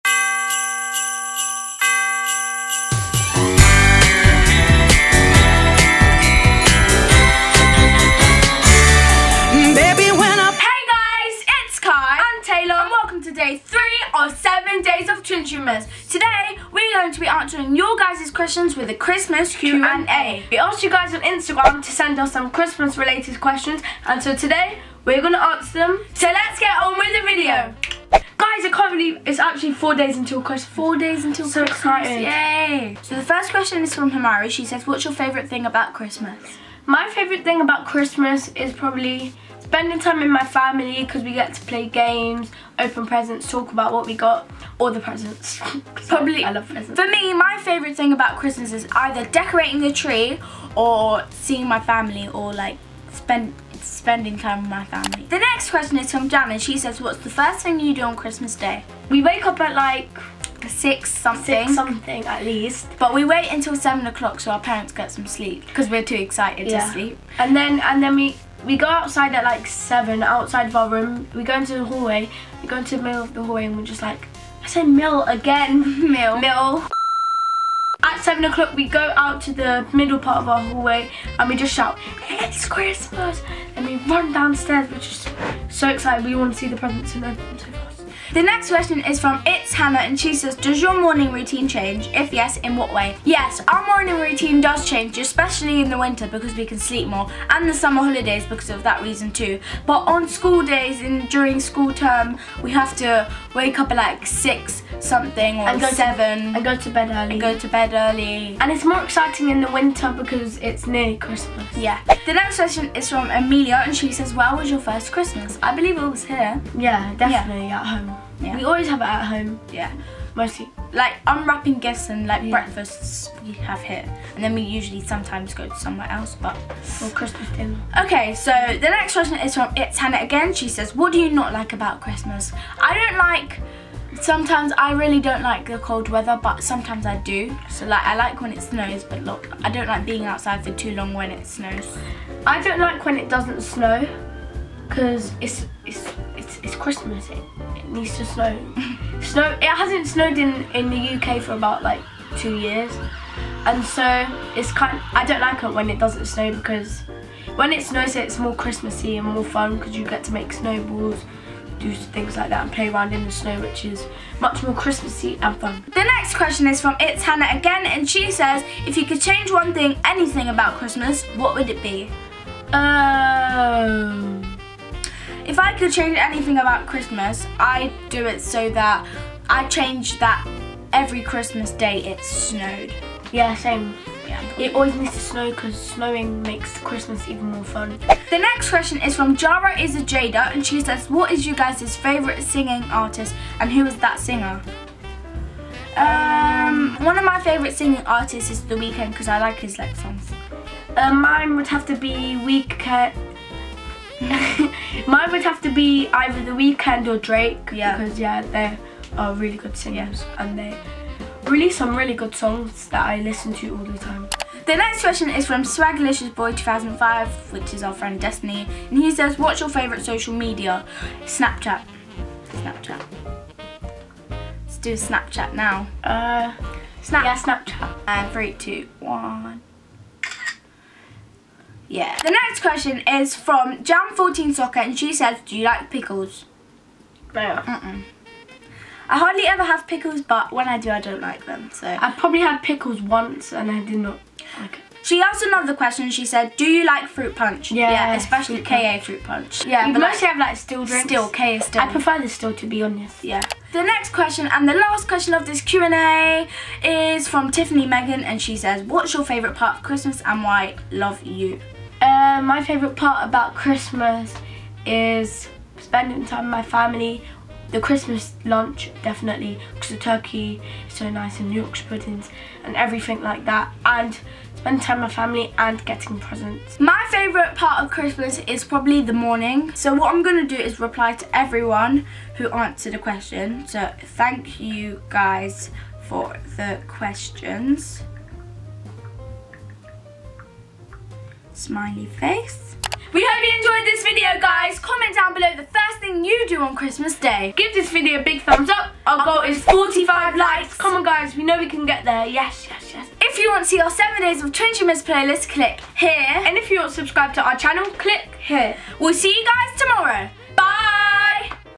XMAS Q&A AskTwintube 2016 | 7 Days Of Twintube-Mas (Day Three)
Hi guys, so for today's video we decided as christmas is coming up, to do a XMAS AskTwintube! We hope you enjoy the video and check out the video to see if your question was answered.

QOTV: What's the first thing you do on Christmas Day?

Social Medias:
Musical.Ly- @twintubeofficial
Personal Musical.Ly's: @Taylor.k__ & @itskaik12

Peace Out Twinners!
-Kai & Taylor